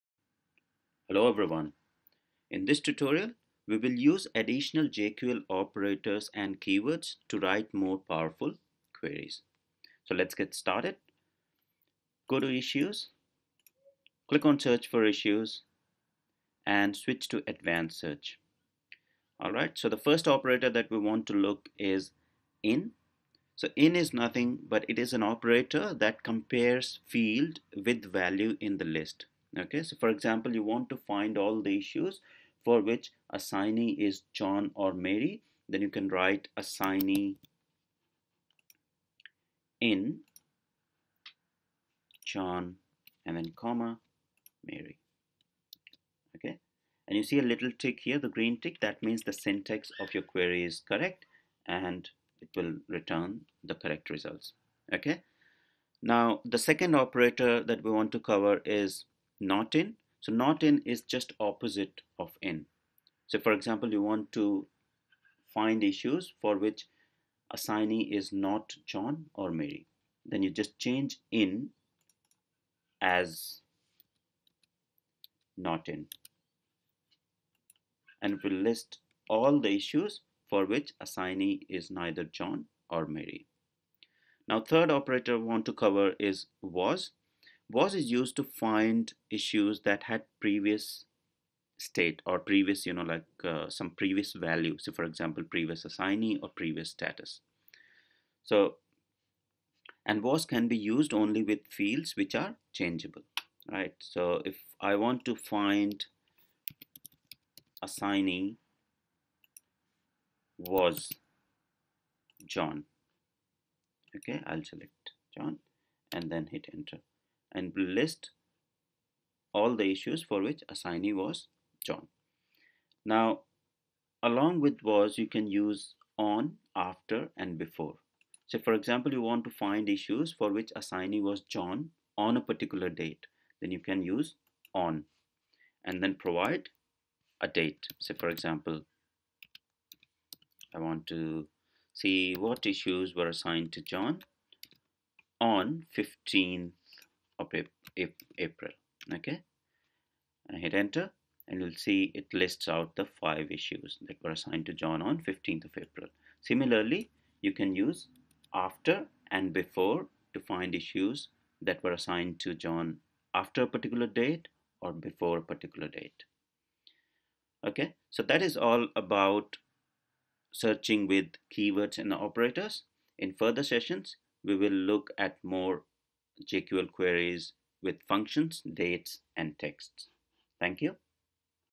JIRA Tutorial #26 - How to Search Issues with JQL Keywords & Operators
In this JIRA Tutorial we will learn how to search issues with keywords & operators. We will learn how to use JQL operators and keywords to write more powerful queries in Jira.